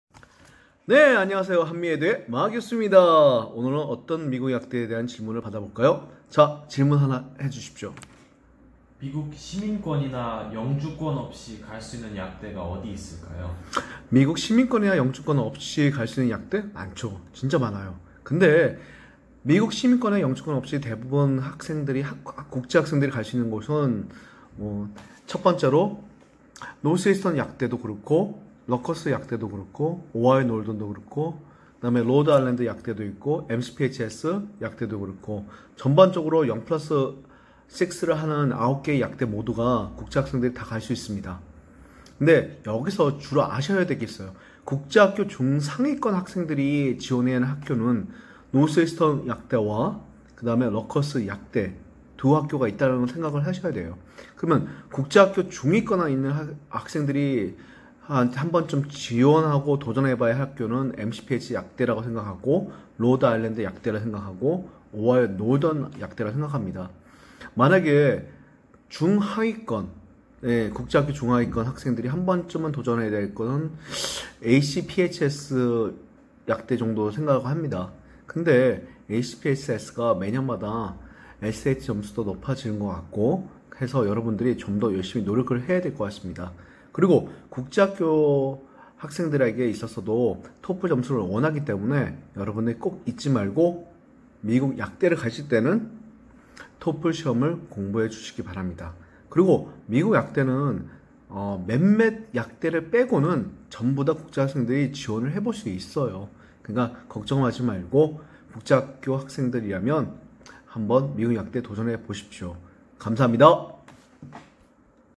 미국약대 ㅡ 미국시민권 ㆍ영주권 없이도 지원할수가 있나요?
0-6 Pharm D 미국 약대들 9개 모두는 국제 학생들을 받아주고 있습니다.

먼저 노스이스턴 약대 합격 합격에 힘들었던 점들을 말해 봅니다.

다음과 같은 링크를 참조해 주세요.

두번쨰로 미국 뉴저지의 명문 약대 럿거스 합격에 대한 SAT/ACT 프로파일에 대해서 말해봅니다.

그리고 실제 미국 약대는 미국 시민권.영주권 없이도 0 + 6 PharmD 9 개의 대학들은 다 지원이 가능합니다.

1) Northeastern University
2) Rutgers University
3) University of Rhode Island
4) Ohio Northern University
5) University of Findlay
6) ACPHS
7) MCPHS University
8) St.John's University
9) PCP

가 있습니다. 감사합니다.
약대 진학 문의는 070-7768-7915 한미에듀 박실장님께